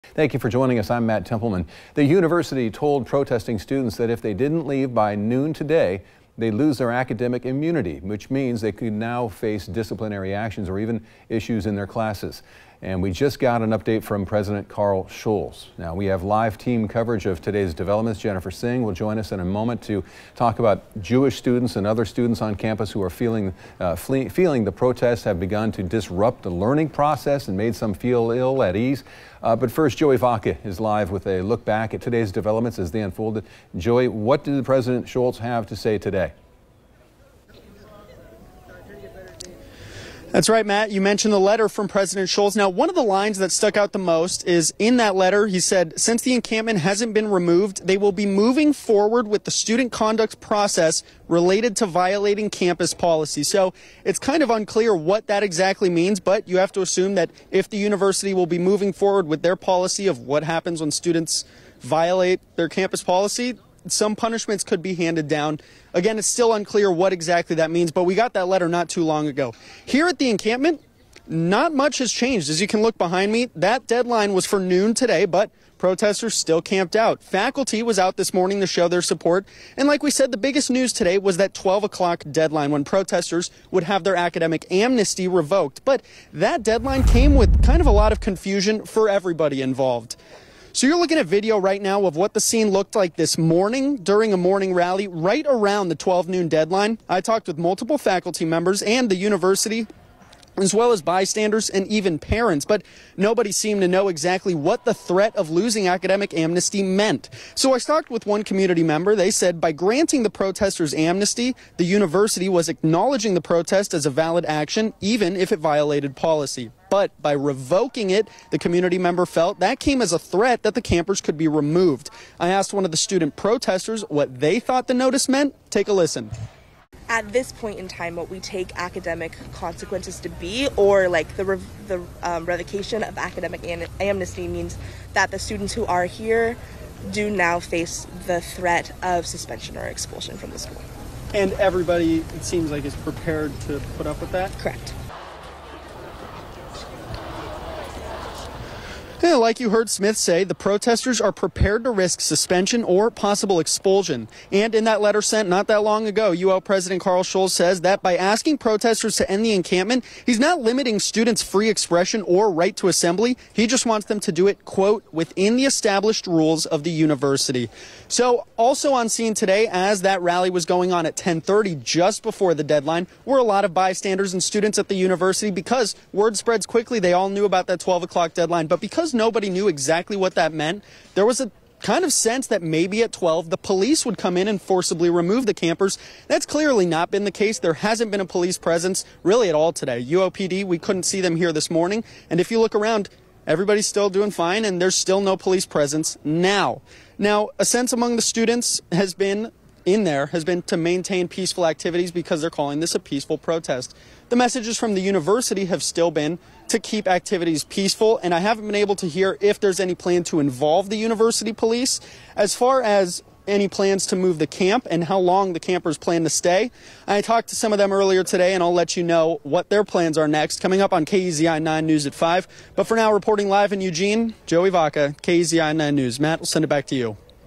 Protestors remain on UO campus after deadline to leave passes
The University notified protestors that if they did not leave by 12:00 p.m. on May 7, the students would have their academic amnesty revoked.